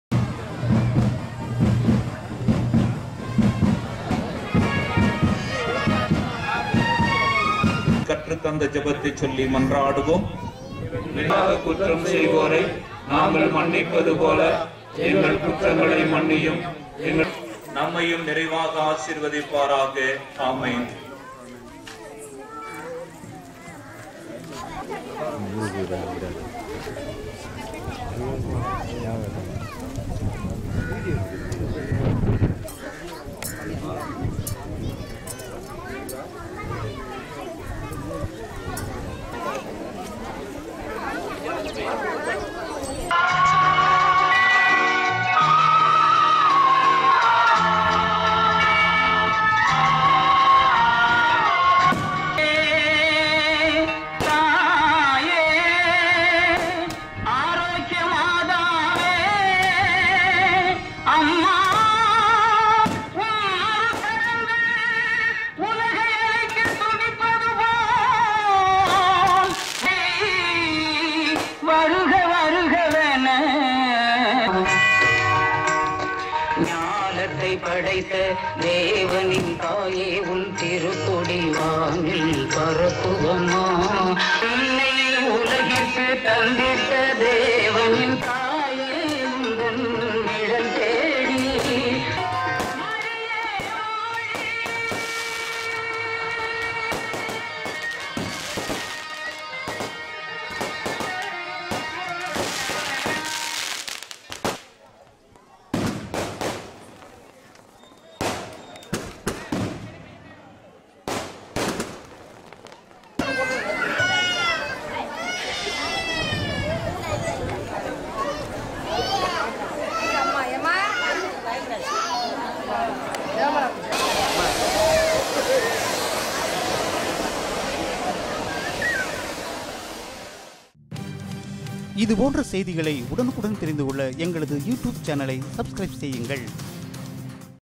புனித அன்னமாள் 70 ஆம் ஆண்டு விழா கொடிஏற்றம் | Madha kovil kodiertram
புதுக்கோட்டை திருவப்பூர் புனித அன்னமாள் ஆலய 70 ஆம் ஆண்டு விழா கொடிஏற்றத்துடன் இன்று தொடங்கியது.
புதுக்கோட்டை திருவப்பூர் அன்னாமாள் புரத்தில் உள்ள  புனித அன்னமாள் தேவாலயத்தின் 70 ஆம் ஆண்டுவிழா வருகின்ற 25 ஆம் தேதி தேரோட்டம் நடைபெற உள்ளது அதற்கான கொடி ஏற்றம் விழா இன்று நடைபெற்றது முன்னதாக அருட்திரு ஜேம்ஸ் அருட்தந்தை தலைமையில் கூட்டுதிருப்பலி நடைபெற்று பின்னர் கொடியினை ஆலயத்திலிருந்து திருவப்பூர் முக்கிய வீதி வழியாக ஊர்வலமா எடுத்துச்சென்று பின்னர் ஆலயம் வந்தடைந்து பின்னர் வன்ன வானவேடிக்கையுடன் கொடியெற்றம் நடைபெற்றது.இதில் ஏராலமான கிருஸ்த்துவர்கள் கலந்துகொண்டனர்.இதனை தொடர்ந்து திருக்கொடியேற்ற திருப்பலி நடைபெற்றது